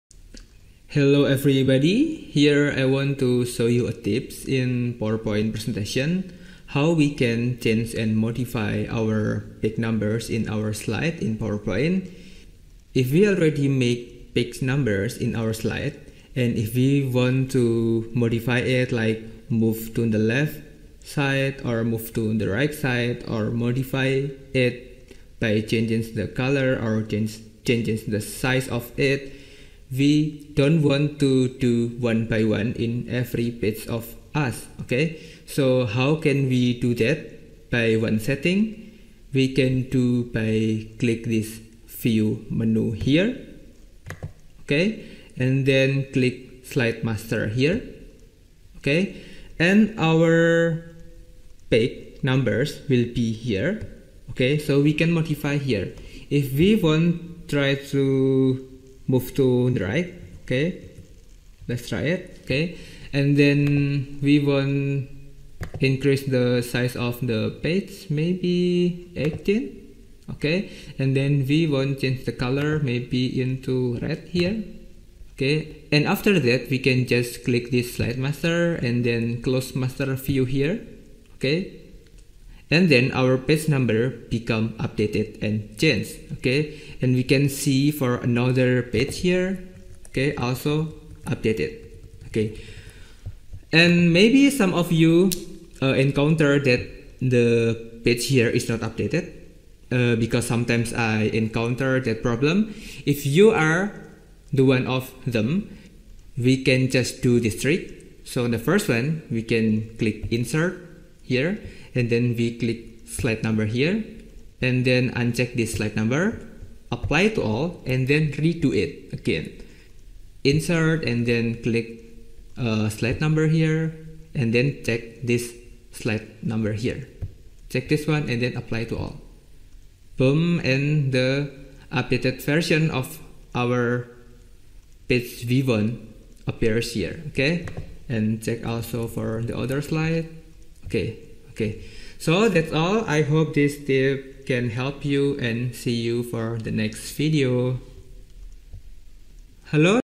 Move, Change Color & Edit Page/Slide Number in PowerPoint
Move Page/Slide Number in Power Point
Change Color Page/Slide Number in Power Point
Change size Page/Slide Number in Power Point
Edit Page/Slide Number in Power Point